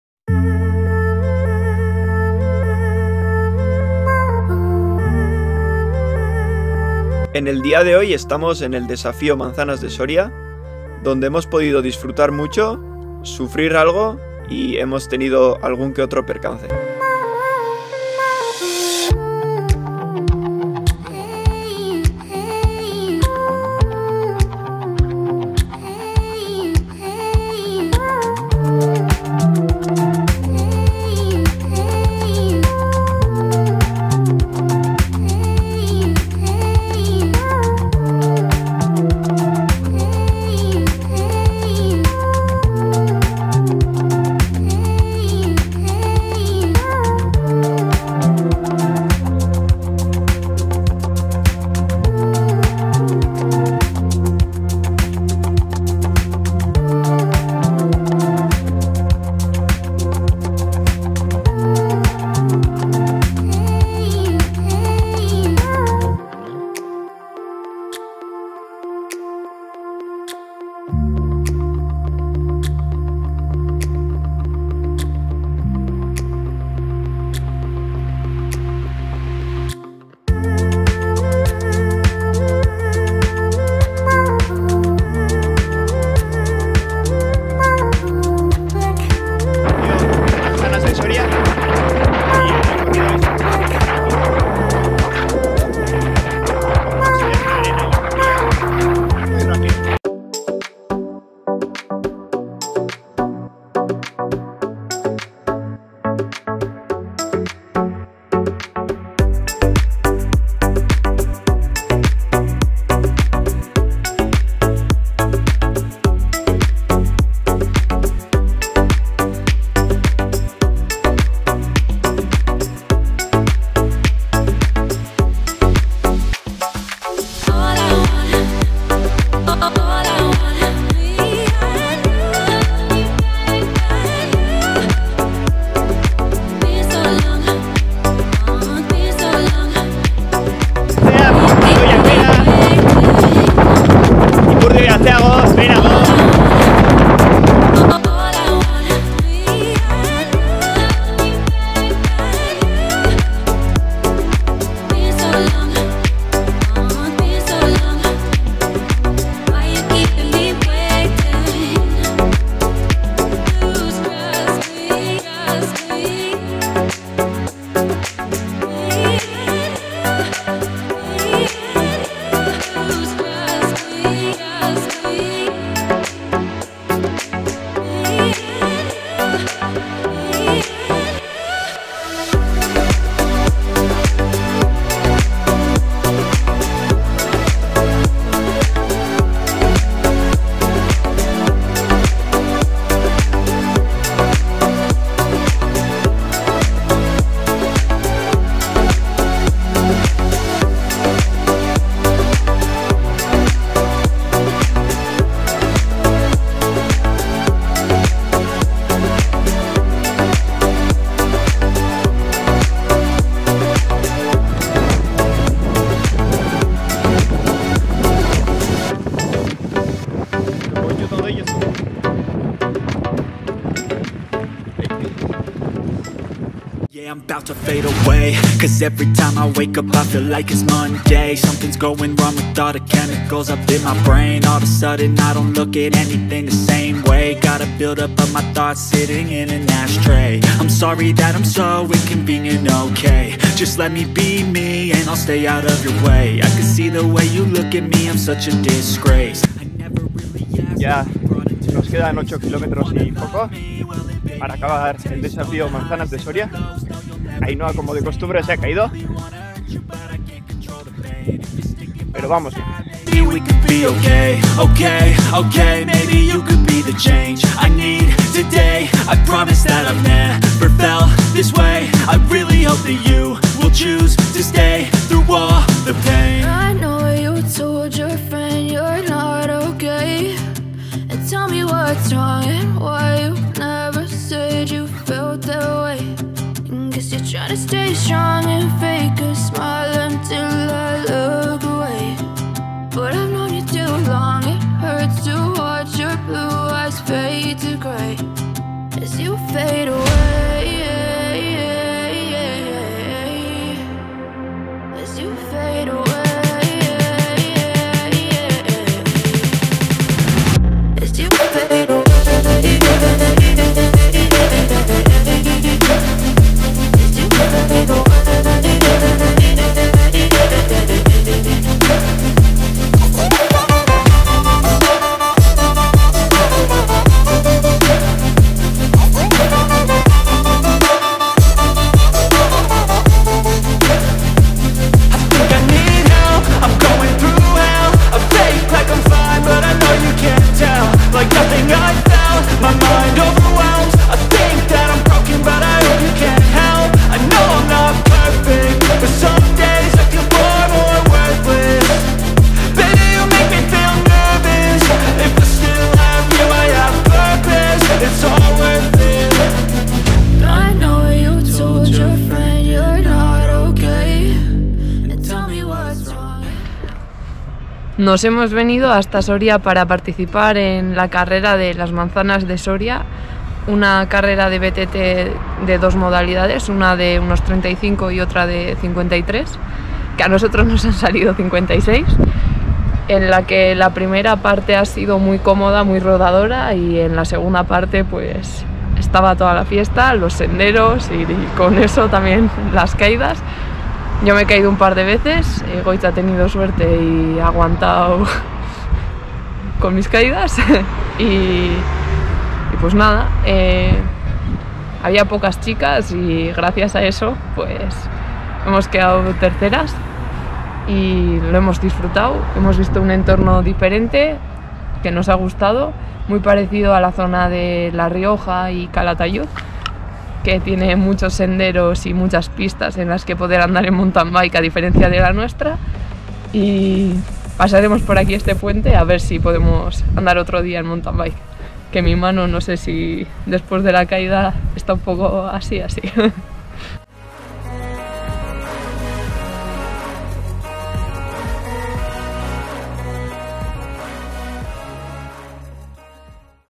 II. DESAFIO BTT MANZANAS DE SORIA - 53KM
Esta vez nos desplazamos a la localidad soriana de El Burgo de Osma para participar en el II.Desafio BTT Manzanas de Soria. Un recorrido muy rápido, donde se junta toda la parte técnica en la zona final, donde Ainhoa se cae dos veces...